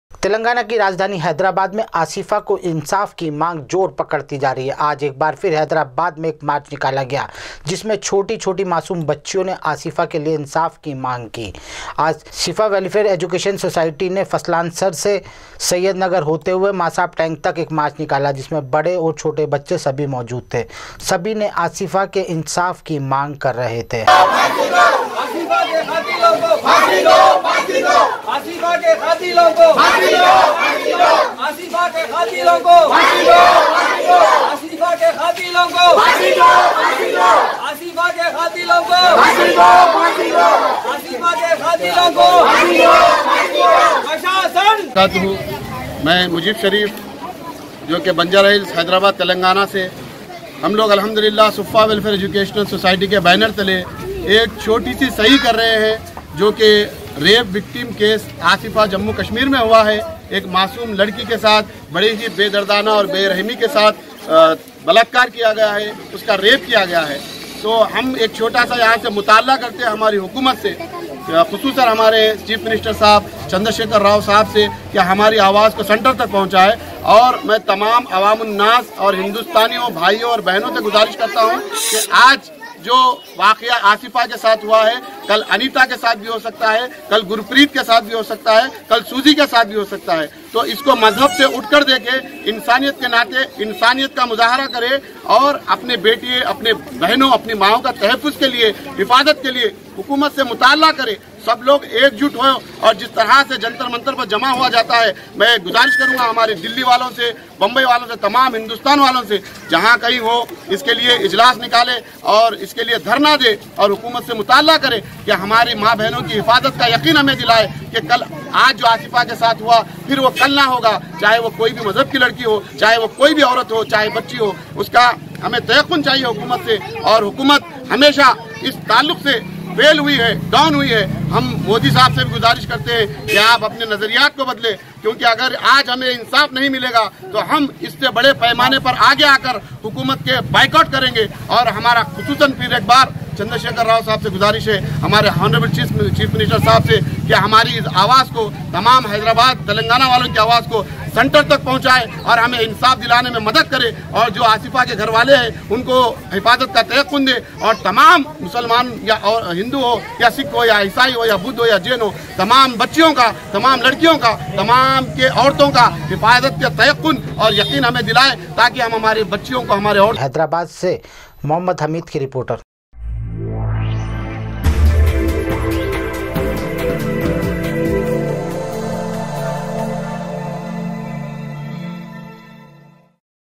Asifa's demand for justice in Hyderabad is gaining momentum THE NEWS INDIA
हैदराबाद में आसिफा के  इंसाफ की मांग जोर पकड़ती जा रही है आज भी आज भी हैदराबाद में एक मार्च निकाला गया जिसमें छोटी छोटी मासूम बच्चियों ने आसिफा के लिए इंसाफ की मांग कीसफा वेलफेयर एजुकेशन सोसाइटी फ़ास्ट लांसर सैयद नगर से से मार्च निकाला गया जिसमें बड़े और बच्चे  थे